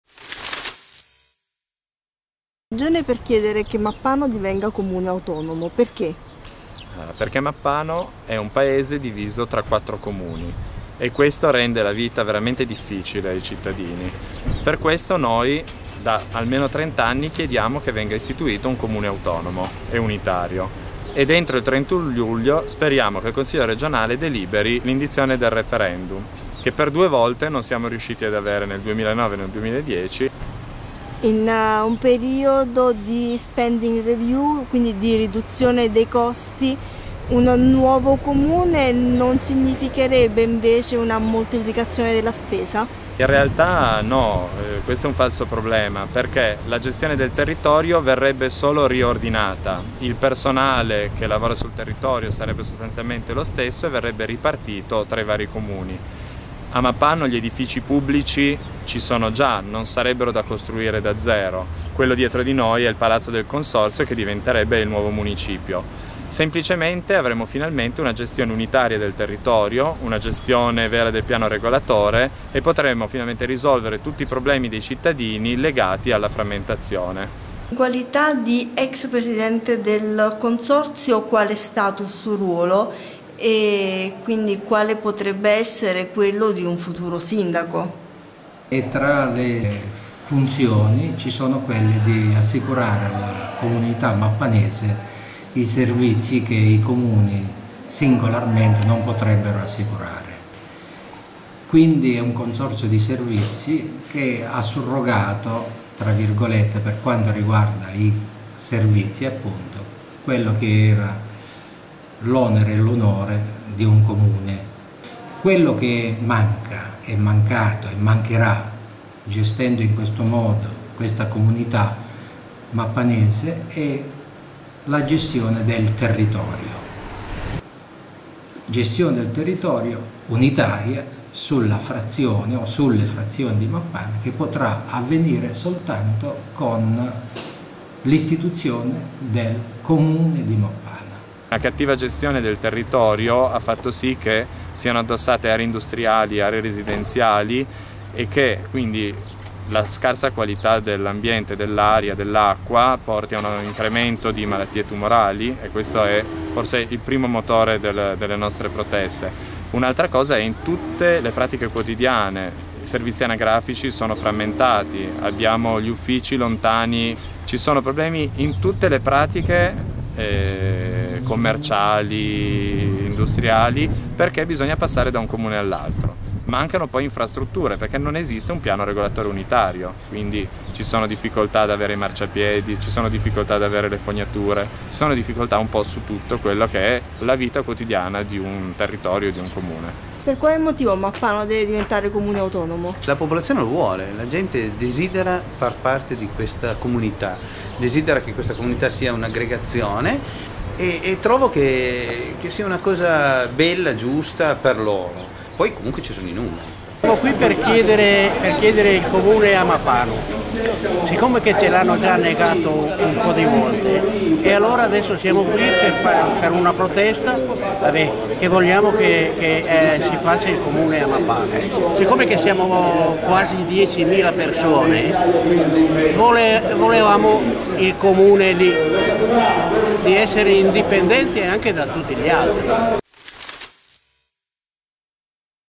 Mappano, il paese diviso in 4
Oltre ottomila abitanti in un paese che sulla carta non esiste. Mappano, periferia di Torino, è diviso in 4 comuni. Per gli abitanti, un inferno di burocrazia. Manca un piano regolatore unitario, i servizi hanno regole diverse.